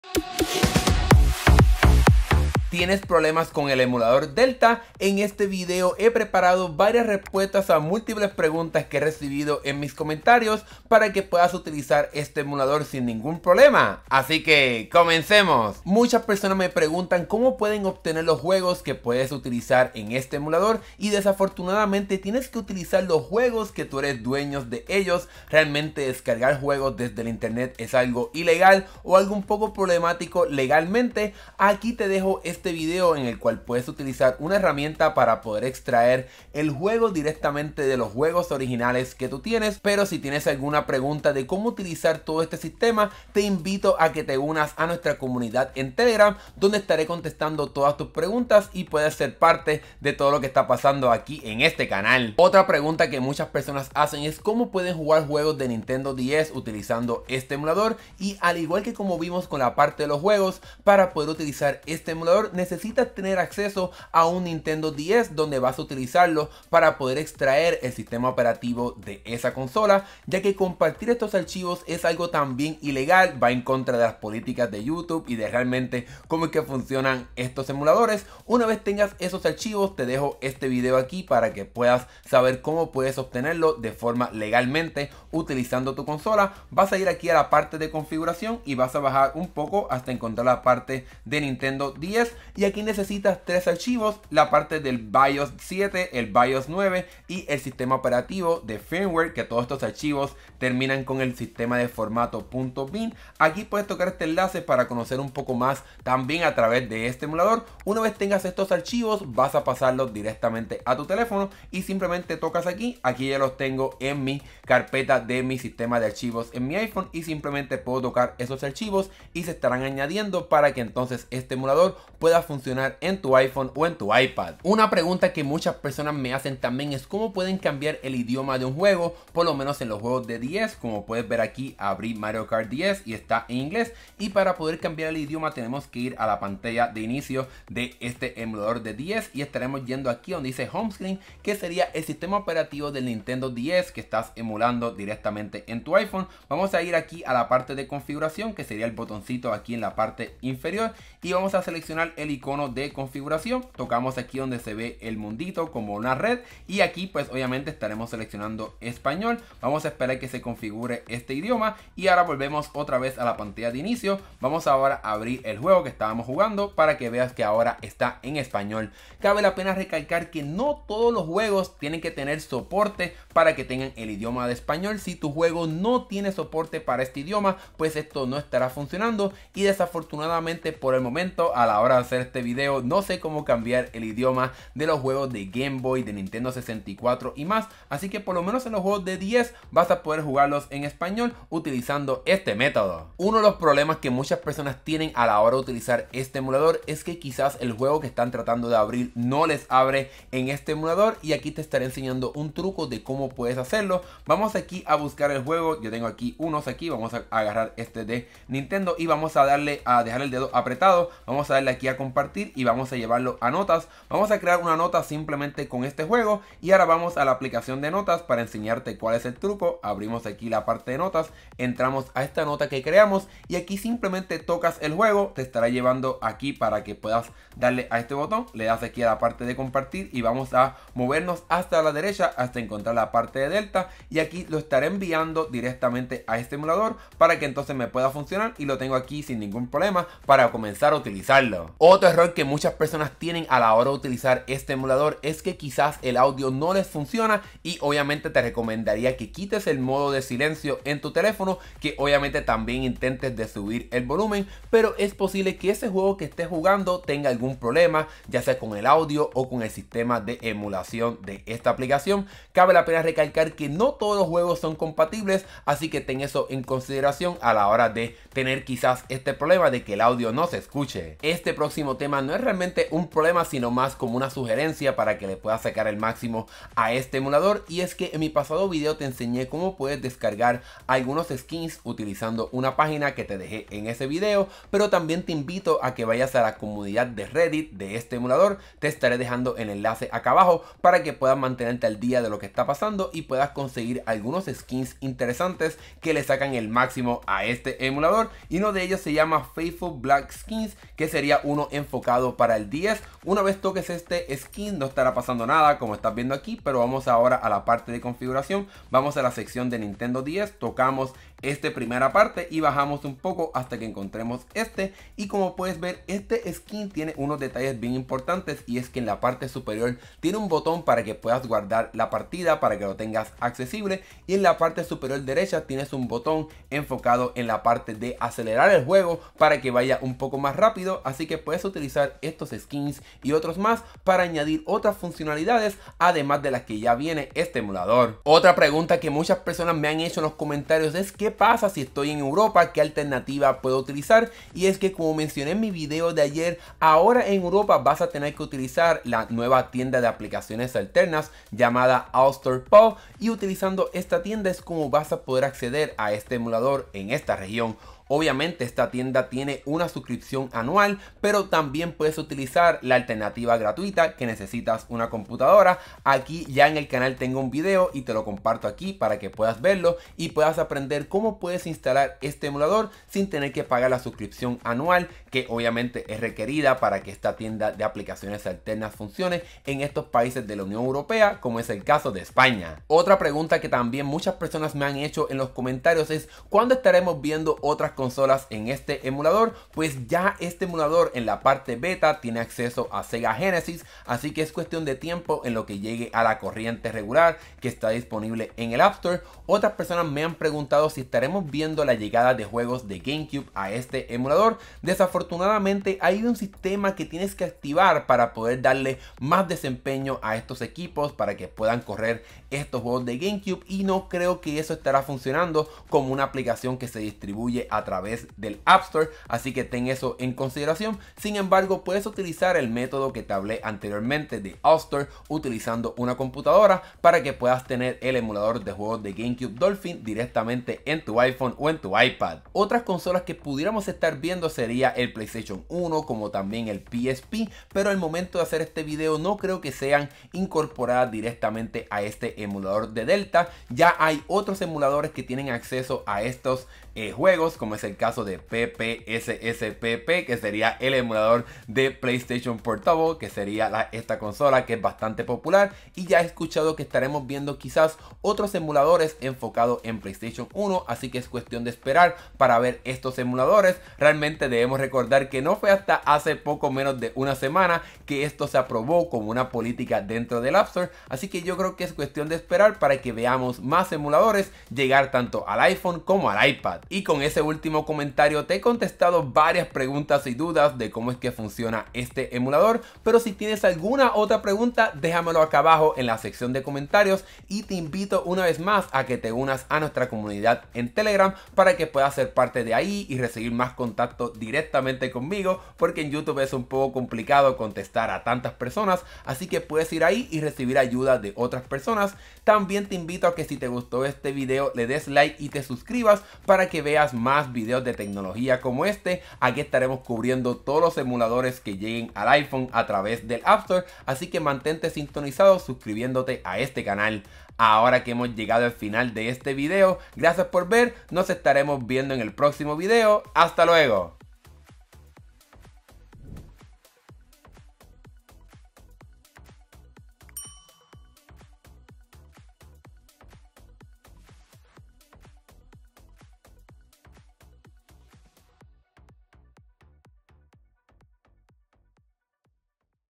Emulador Delta - Preguntas Comunes - ROM, Cómo Jugar Nintendo DS, Cambiar de Idioma y más
En este video te contesto muchas de las preguntas que tienen muchas personas con respecto al emulador de Delta. Si tienes más preguntas, puedes dejar tu comentario en este video.

0:00 - Intro
0:15 - Juegos
0:52 - Jugar Nintendo DS
2:04 - Cambiar Idioma
3:13 - No Abre el Juego
4:02 - No se Escucha
4:31 - Skins Más Útiles
5:28 - Europa - España
6:24 - Otras Consolas